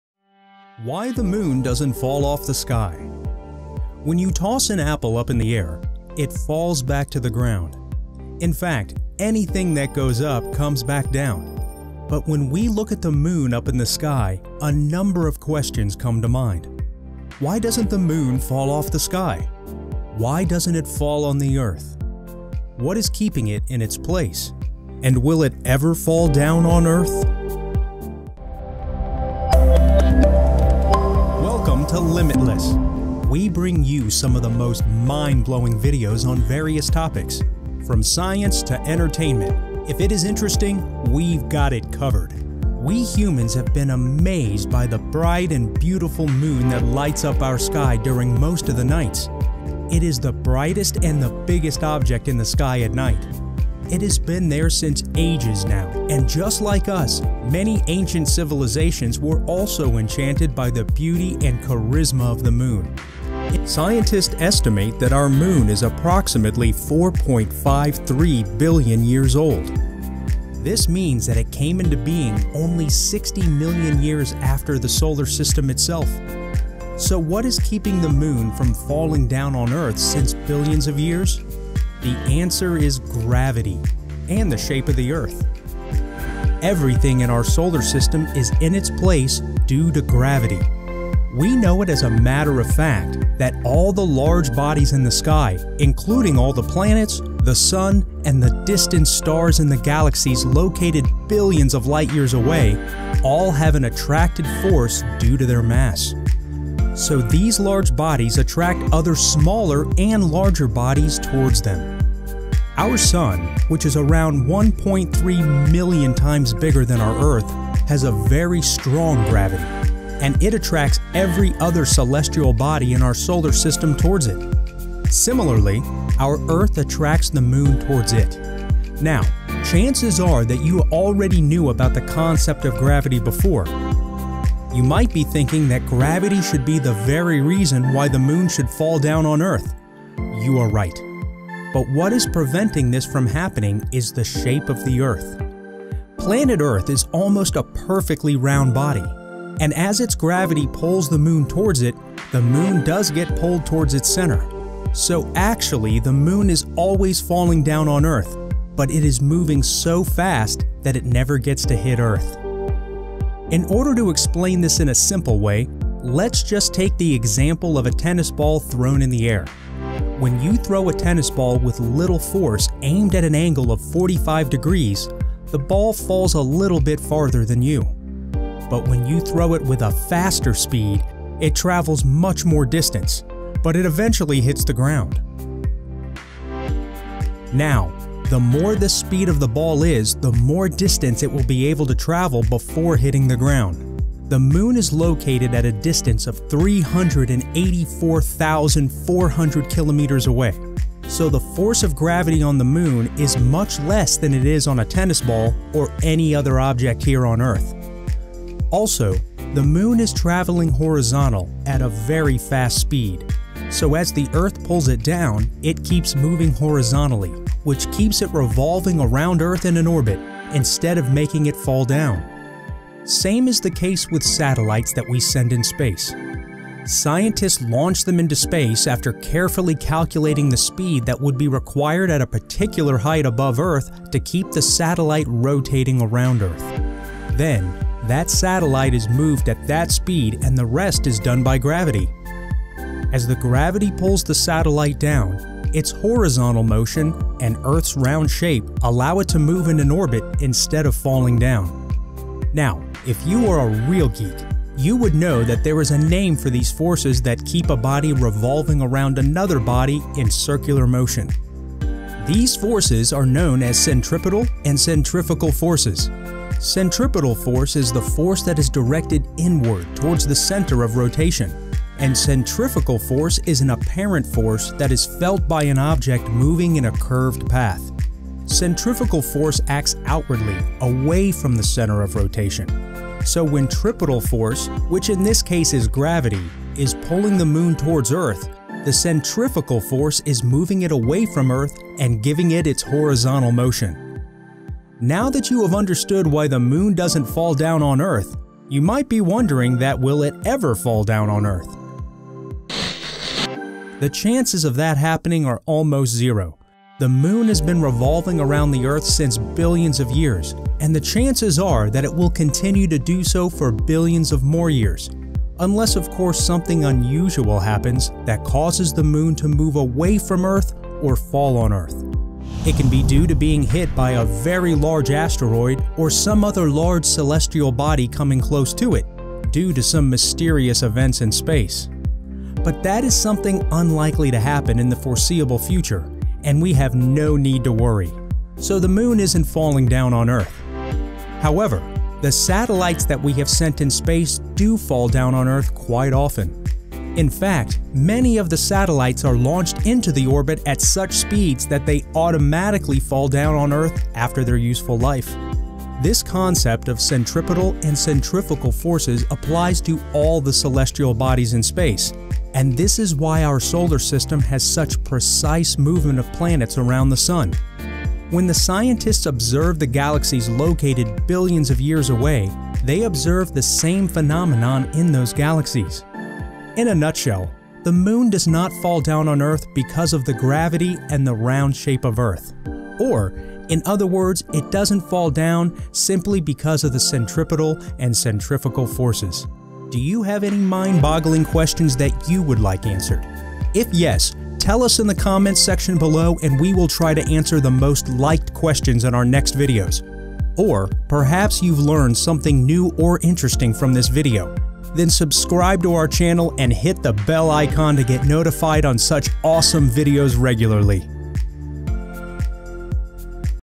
Why Doesnt The Moon Fall Off The Sky
Why Doesn't The Moon Fall Off The Sky? Why doesn’t it fall on Earth?
What is keeping it in its place? And will it ever fall down on Earth?

We, humans have been amazed by the bright and beautiful moon that lights up our sky during most of the nights. It is the brightest and the biggest object in the sky at night. It has been there since ages now, and just like us, many ancient civilizations were also enchanted by the beauty and charisma of moon. In fact, scientists estimate that our moon is approximately 4.53 billion years old. This means that it came into being only 60 million years after the solar system itself existence.
So what is keeping the moon from falling down on Earth since billions of years?
The answer is in this video.